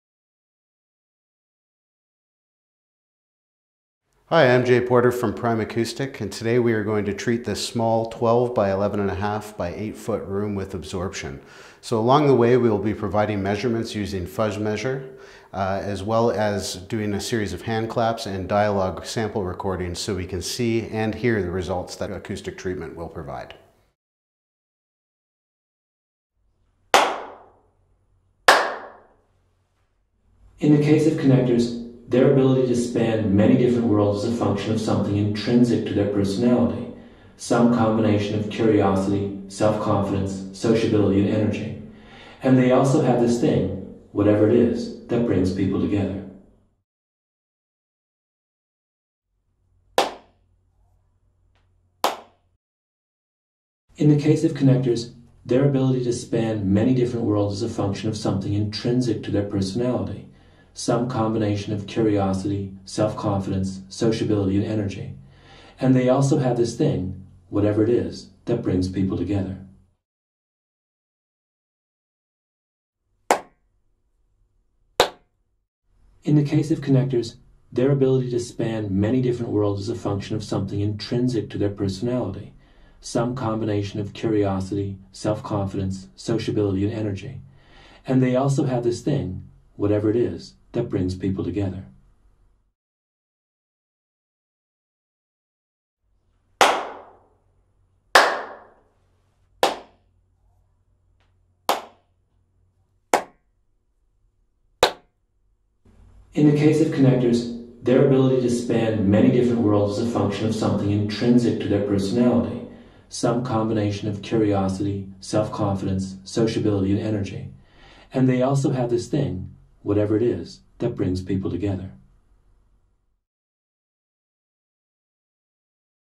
Comparaison avant / après traitement PRIMACOUSTIC en vidéo (La Boite Noire)
Cette vidéo va vous démontrer l'efficacité des panneaux de traitement acoustique PRIMACOUSTIC.

Les panneaux de traitement PRIMACOUSTIC permettent de traiter l'acoustique de votre pièce par absorption du son.